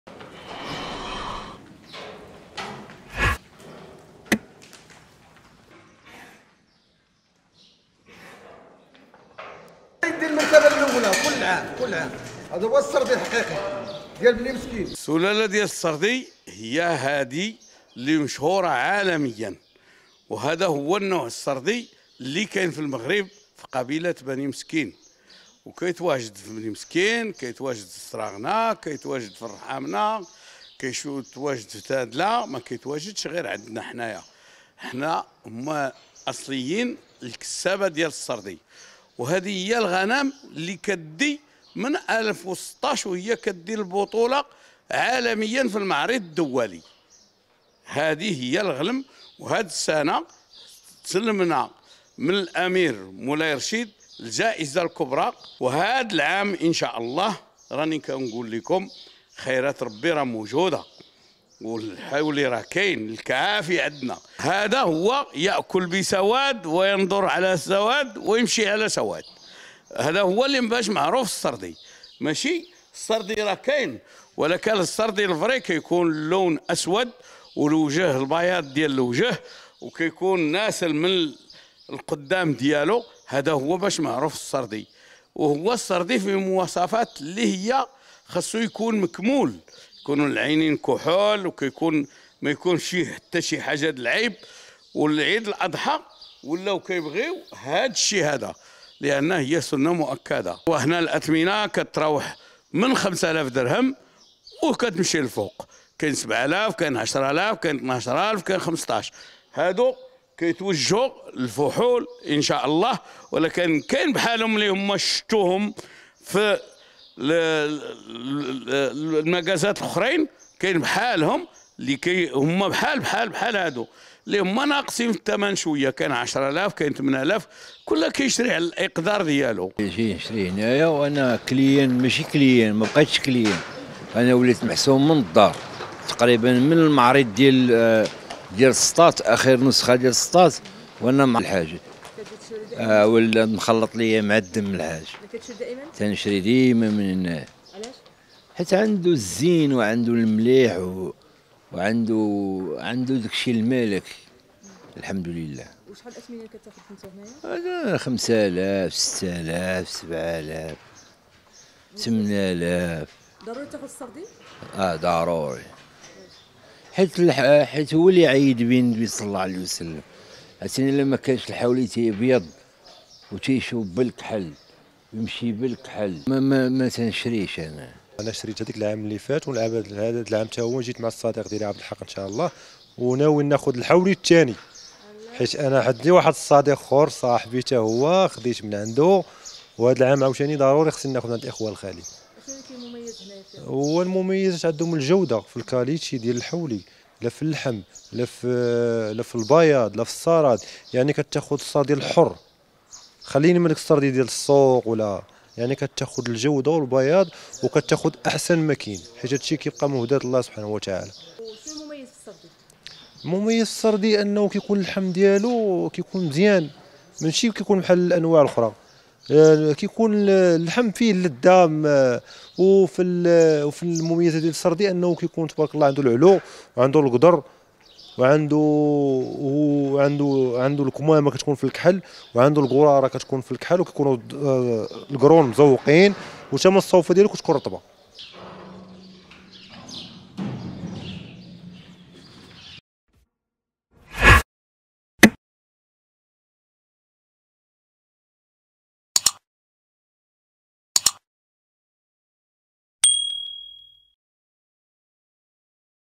لهذا يختار المغاربة خروف الصردي كأضحية للعيد
تعتبر أغنام الصردي من أجمل سلالات الأغنام في المغرب، والتي يكثر عليها الإقبال في عيد الأضحى، لتوفرها على صفات لا توجد في بقية السلالات، حيث تلخص المقولة الشهيرة على الصردي كل صفاته: "يأكل من سواد وينظر بسواد ويمشي على سواد".